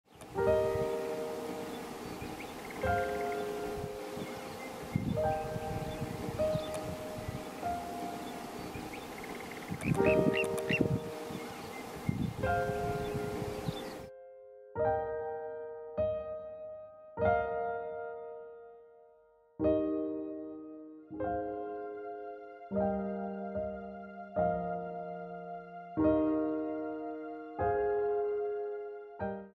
Graceful Posing by the Rapeseed Field, Little Ring Plover
A Miniature Marvel by the Rapeseed field, The Little Ring Plover@discover-photography-vlad​.  A short moment of Mindful photography and Time Outdoors in nature. Canon 90D wildlife photography.